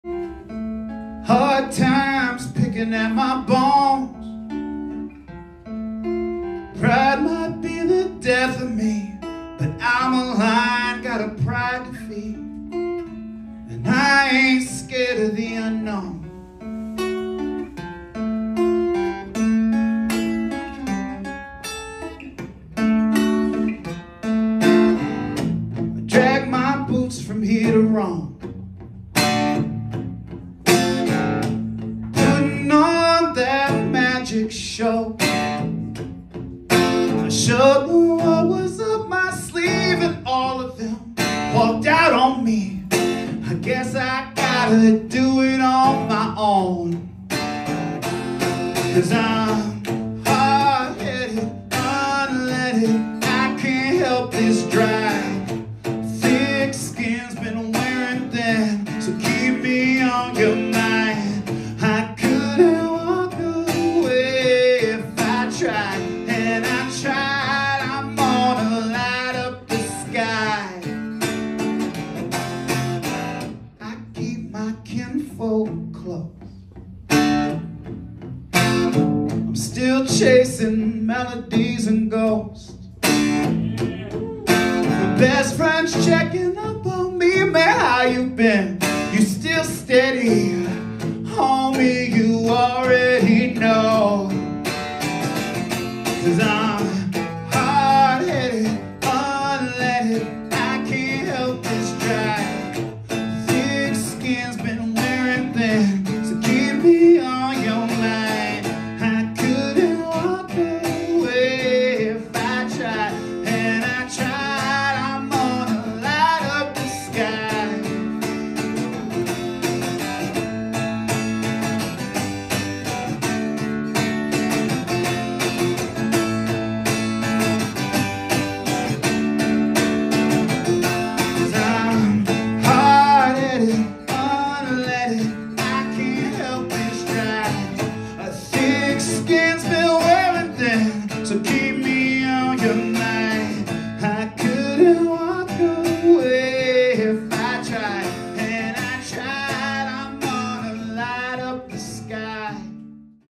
Phillip-Michael Scales still chasing melodies and ghosts
Live at The Ark in Ann Arbor on Feb. 17, 2024.